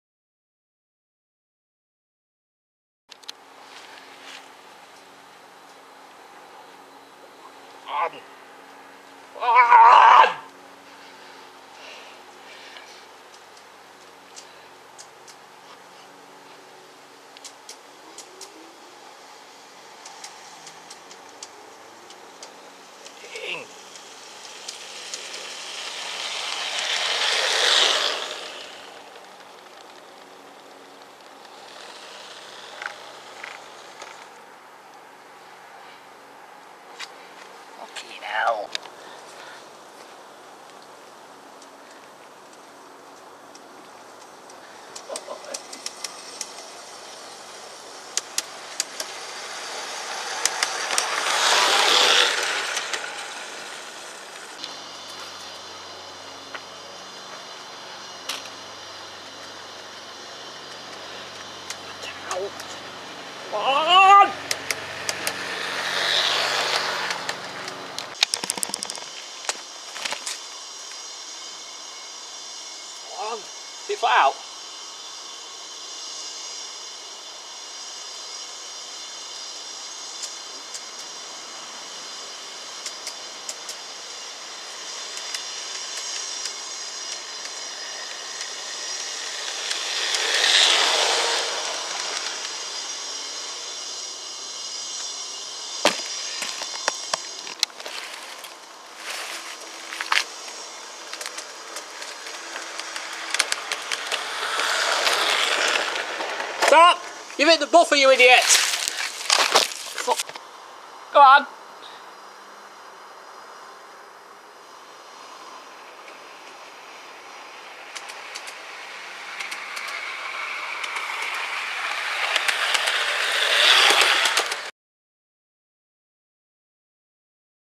Humberston Garden railway Lineside video,21 11 2020
Here is our second video of the half completed garden railway,Running Alco AL-3 Union Pacific 1195.